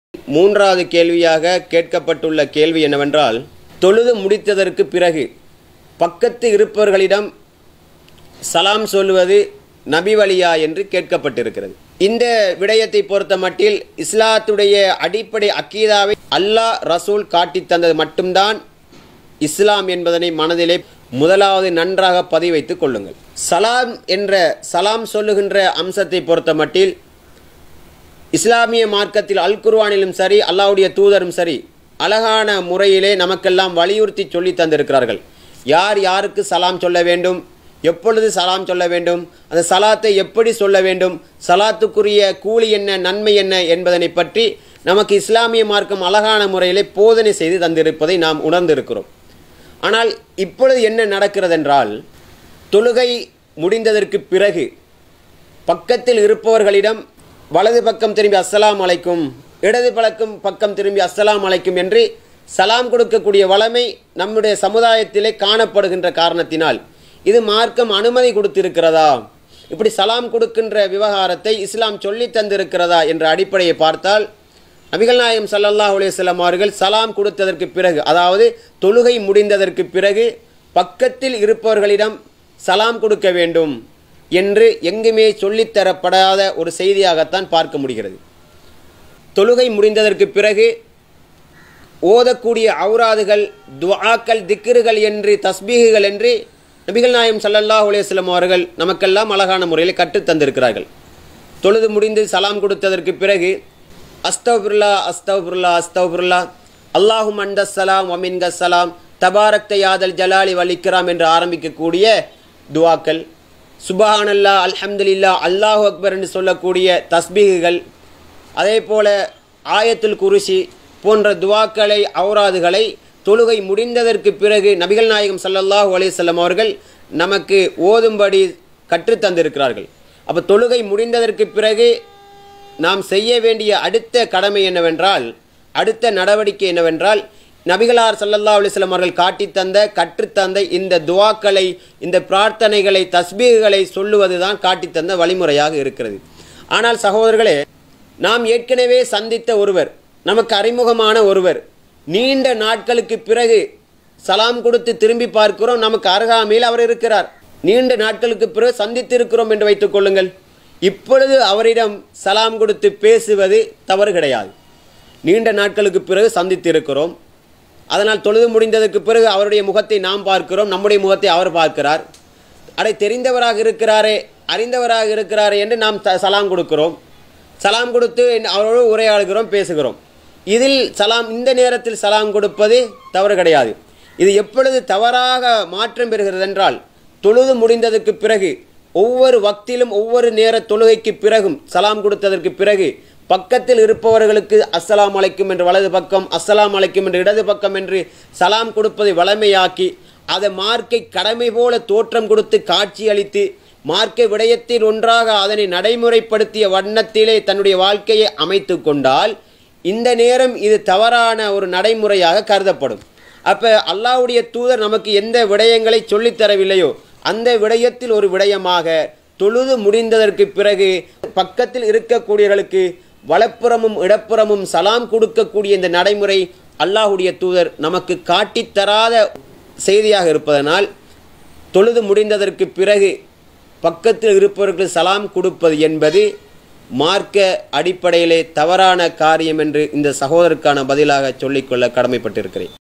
எது நபி வழி
தொழுது முடித்த பின்பு அருகில் உள்ளவருக்கு ஸலாம் சொல்வது நபி வழியா?
(04 Min Video)

தொலைக் கேள்விகளும் துரித பதில்களும் | EP 08 | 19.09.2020

★கேள்விகளுக்கு பதிலளிப்பவர் : H.M.நதீர் மஸ்ஊதி (பேச்சாளர் SLTJ)